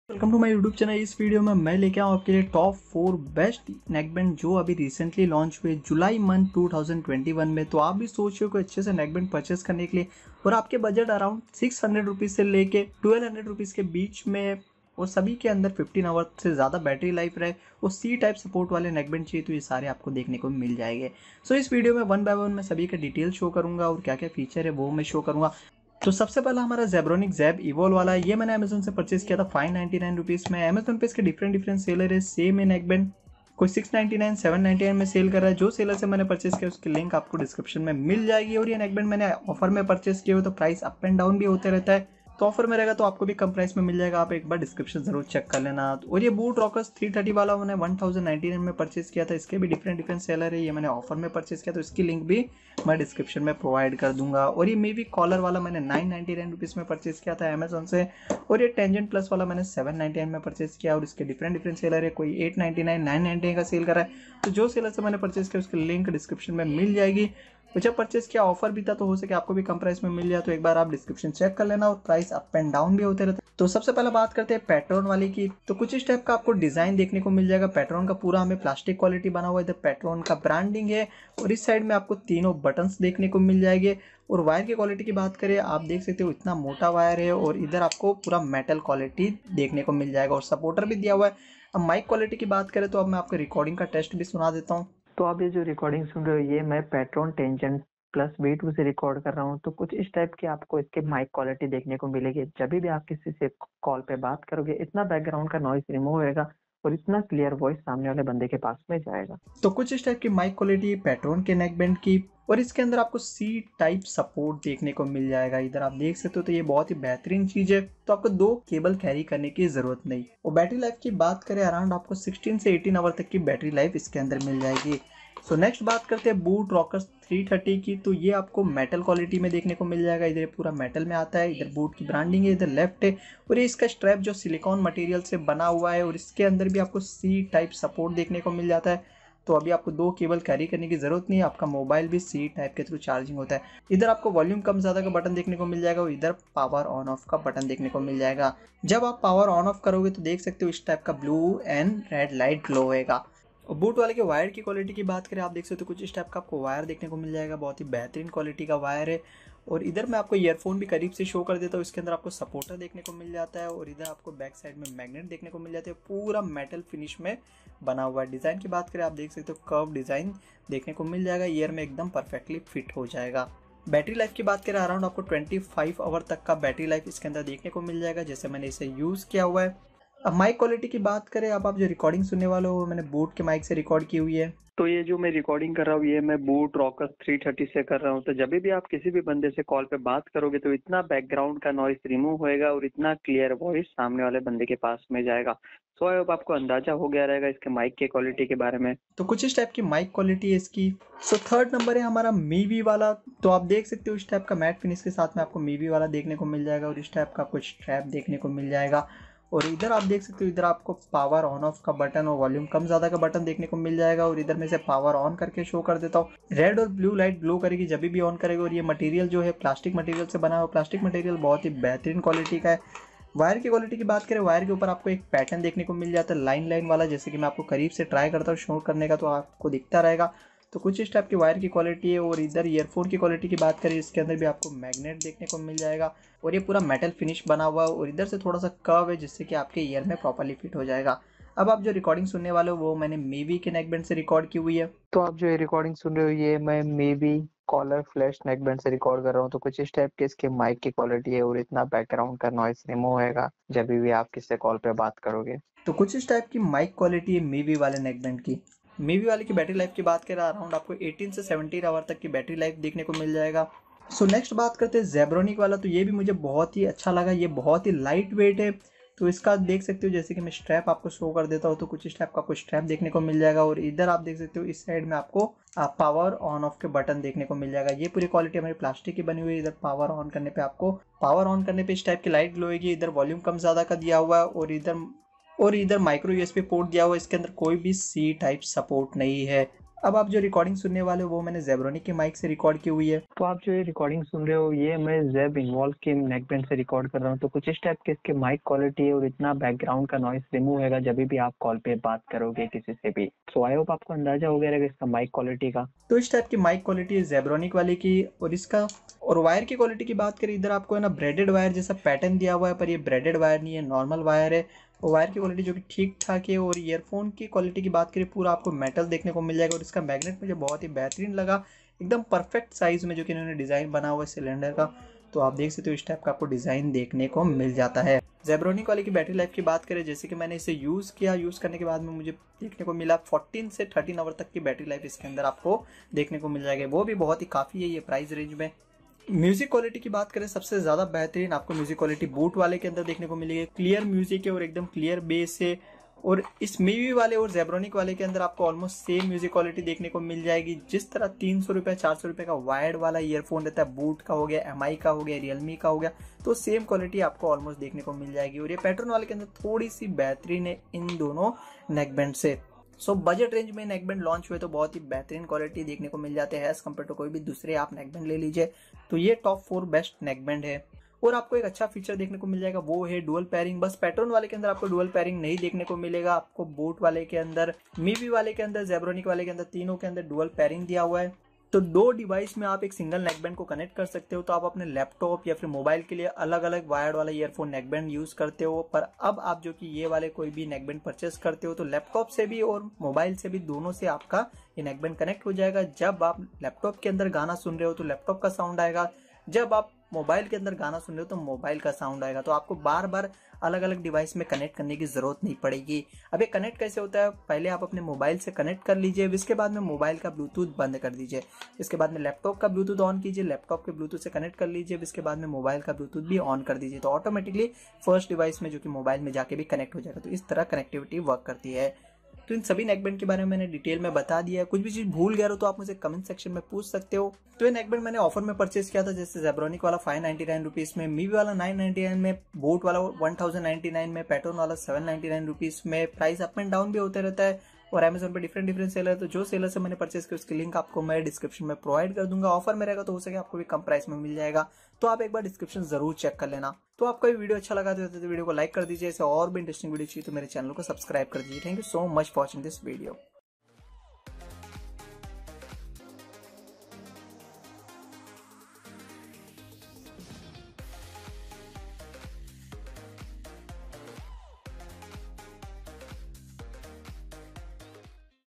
Top Best Bluetooth Neckband Earphones Under Rs.1000 ⚡ July 2021
Mivi Collar Flash Bluetooth Earphones. Fast Charging Wireless Earphones with mic, 24hrs Battery Life, HD Sound, Powerful Bass, Made in India Neckband

boAt Rockerz 330 Wireless Neckband with ASAP Charge, Up to 30H Playback, Enhanced Bass, Metal Control Board, IPX5, Type C Port, Bluetooth v5.0, Voice Assistant(Active Black)

Zebronics Zeb Evolve ​Wireless ​in Ear​ ​Neckband Earphone​ ​with BT 5.0, Rapid Fast Charging, Up to 17H Battery Life, Dual Pairing, Call Function, Magnetic Earpiece, Call/Media Controls

pTron Tangent Plus V2 with 18Hrs Playback & Type-C Fast Charging, Bluetooth 5.0 Wireless Headphones with Deep Bass, IPX4 Water Resistance, Snug-fit, Voice Assistance & Built-in Mic

🖥️ Led Monitor
💻 Laptop which I use for Video editing
🎤Boya Microphone
🎤Another Blue Snowball iCE USB mic